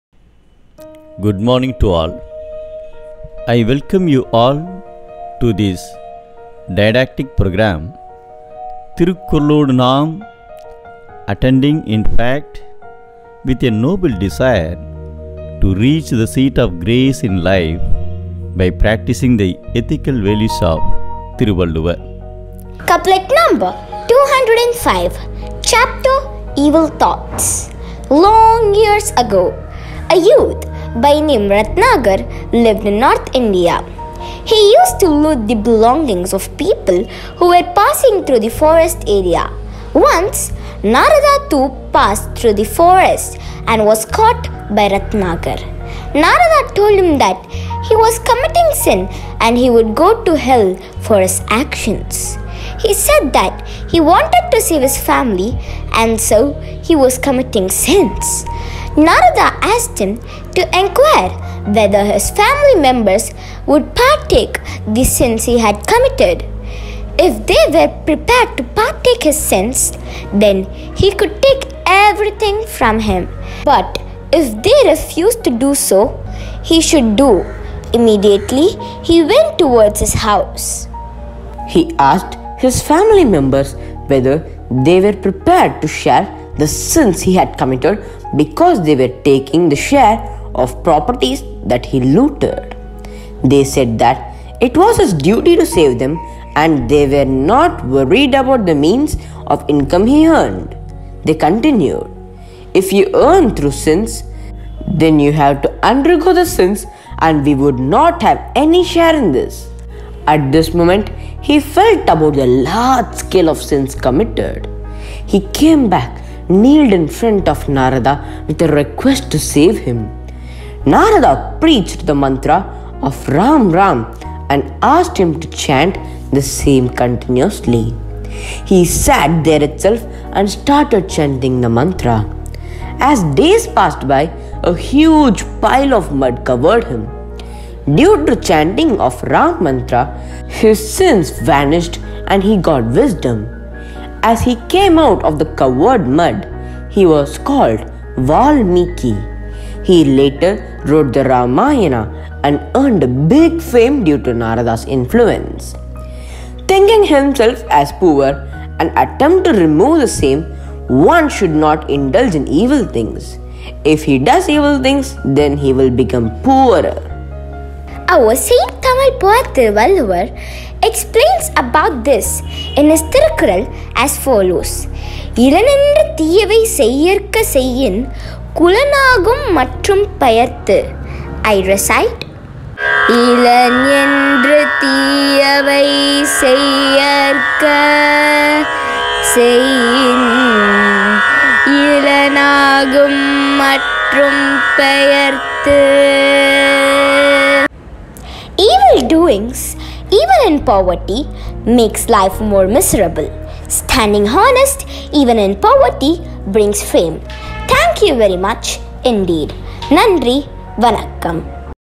Couplet- 205, Chapter- Evil Thoughts: "Thirukkuralodu Naam"
Couplet 205:
Make not thy poverty a plea for ill;
Thy evil deeds will make thee poorer still.

Couplet Explanation:
Commit not evil, saying, "I am poor": if you do, you will become poorer still.